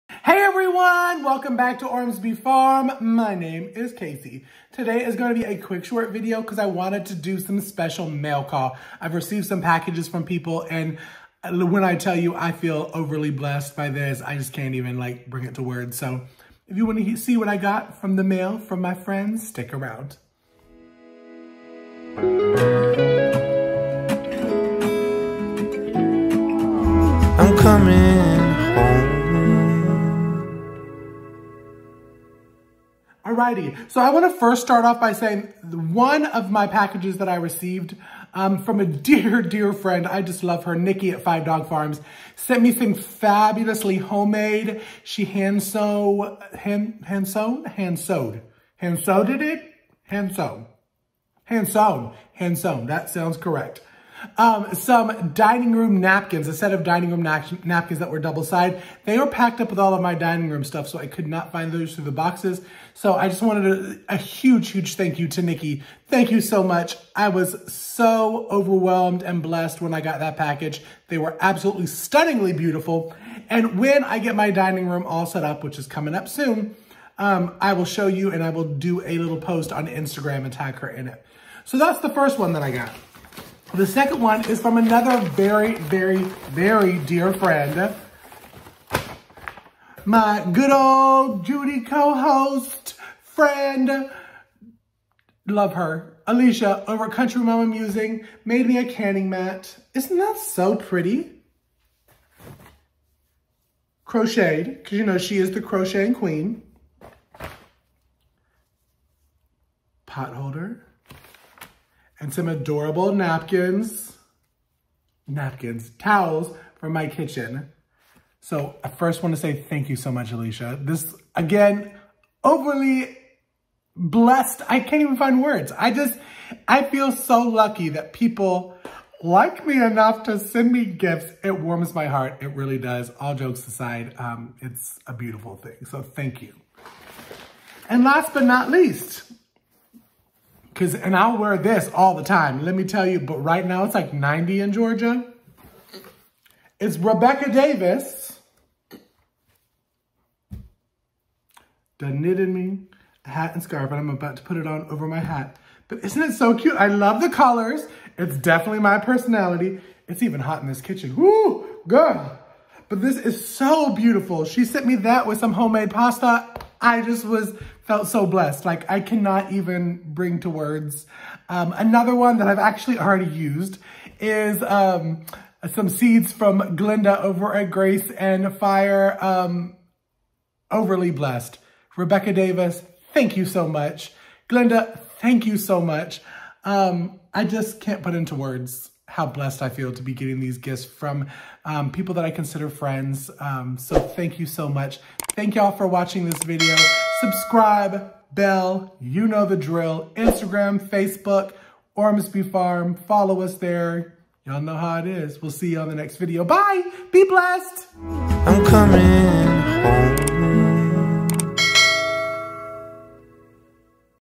First MAIL CALL // Thanks for the beautiful gifts!
A HUGE HUGE thanks to the subscribers who have become friends and sent me beautiful gifts!!!
@arusticrenaissance - Thanks so much for my beautiful dinner napkins
@countrymamamusings7701  - Thanks SOOOO much friend for my canning mat and more!
@graceandfire - Thank you SOOOOO much for the act of kidness of the seeds!
RebekahDavis - Thank you SOOOOO much for my hat and scarf

The Guest Family Amazon List

Ball Canning Book

Our Equipment
Camera :
Microphone:
Drone:
Selfie Stick/tripod: